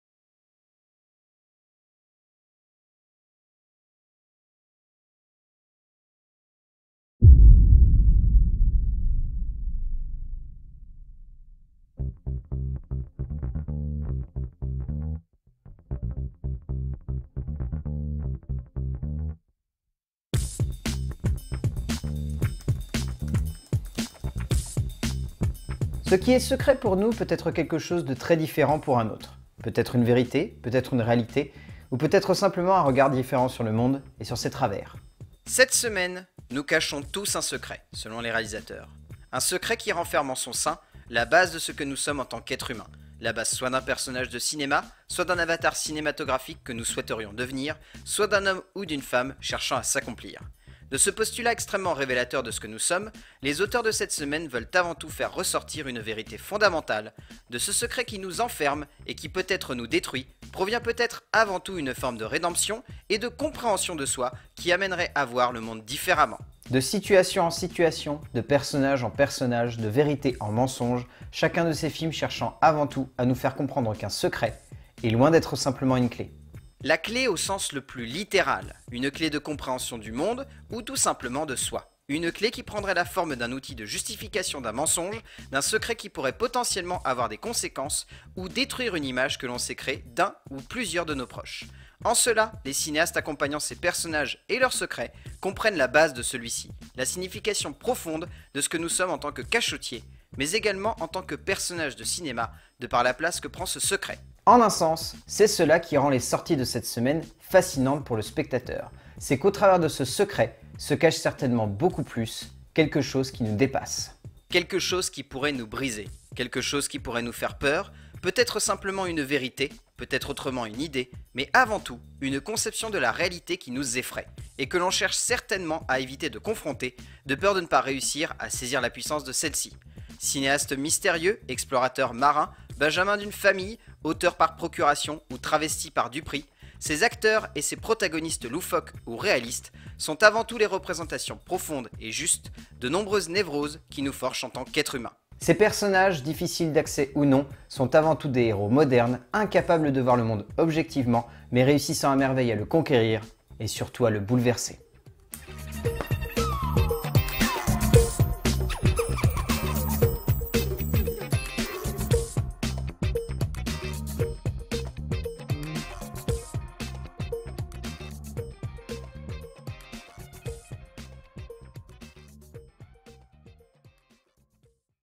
Edito 07/03/18 : Secret et face cachée
Editorial revenant sur le fil conducteur des sorties de cette semaine, le 7 Mars 2018 : Secret et face cachée